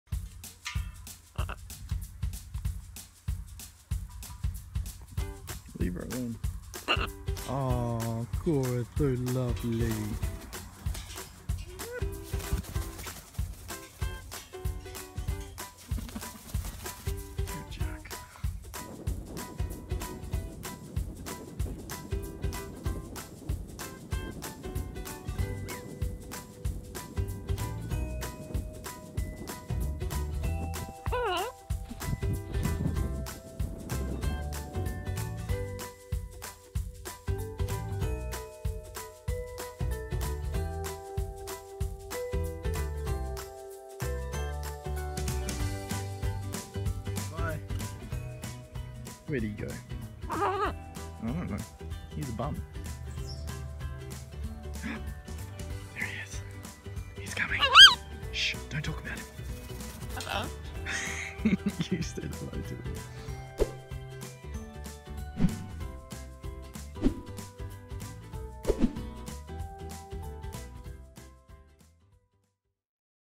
Now That's a Birds Eye View: Amazing Friendship Between Man And Parrot
A dedicated bird lover has struck up such an amazing bond with his parrots he’s able to record how they see the world when they take flight.

Brody Murray, from Brisbane, Australia, owns two magnificent birds – eight-month-old green-winged macaw Korra and three-year-old blue and gold macaw Roku

As this amazing footage shows the three of them love nothing better than getting outdoors and going for parrot walkies – or flighties.

Filmed By: Brody Murray
Sourced By: Richard Ashmore
Edited By: Keiran Fuller